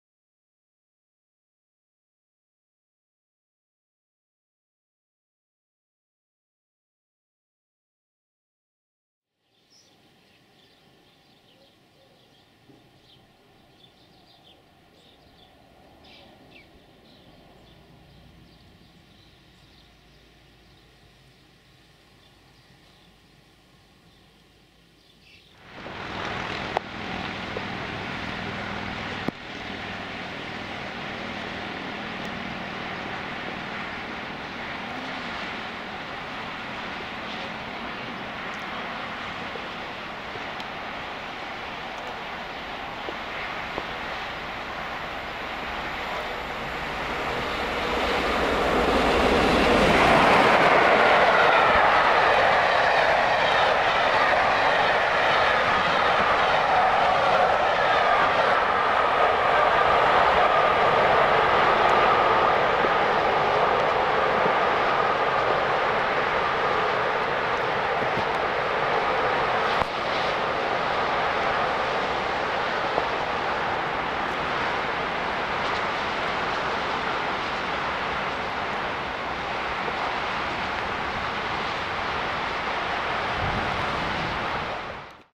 60163 Tornado ,The Torbay Express , 12th July 2015
60163 Tornado races through the pouring rain between Whiteball and Tiverton with the Torbay Express from Bristol Temple Meads to Kingswear .